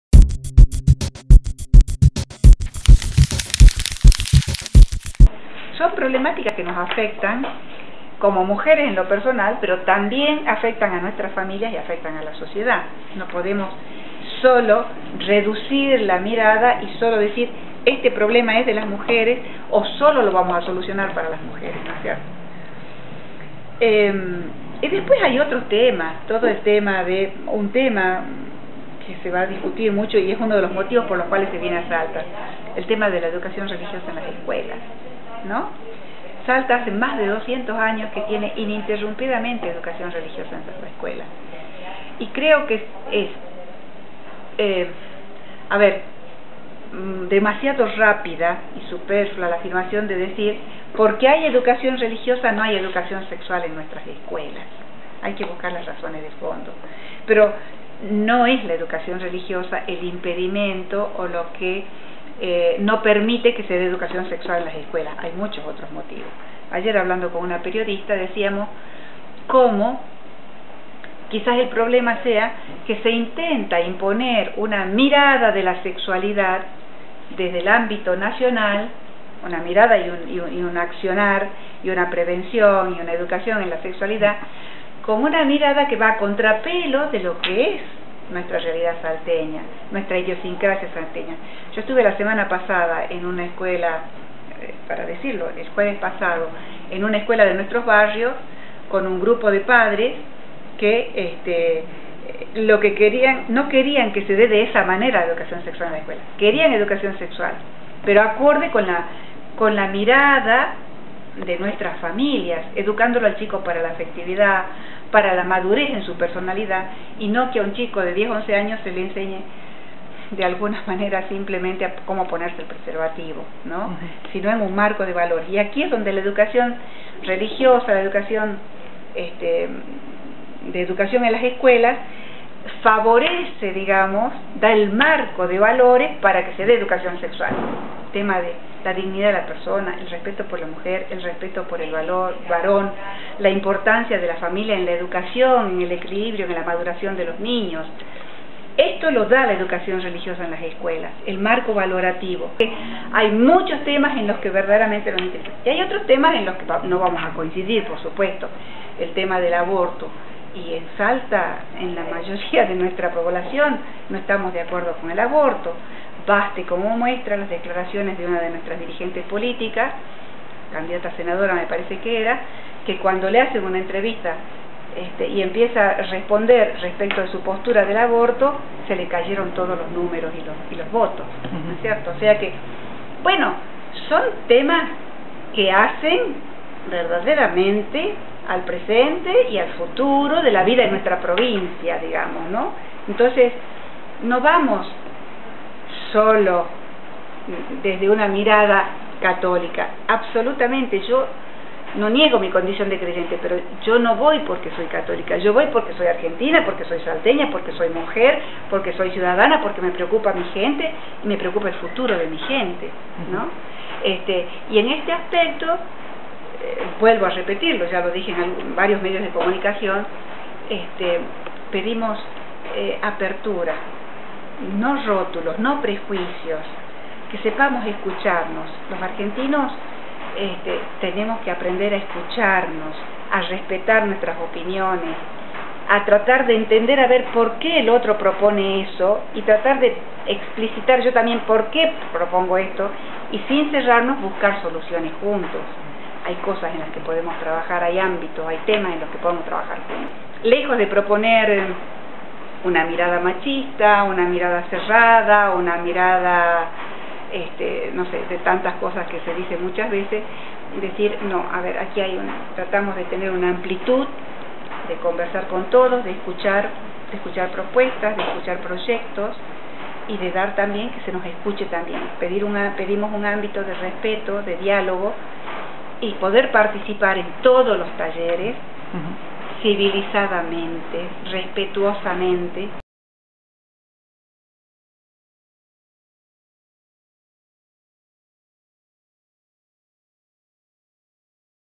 Encuentro de mujeres (2da parte)
Entrevista a Rosa Zacca - Directora del Instituto de la Familia y la Vida, Universidad Católica de Salta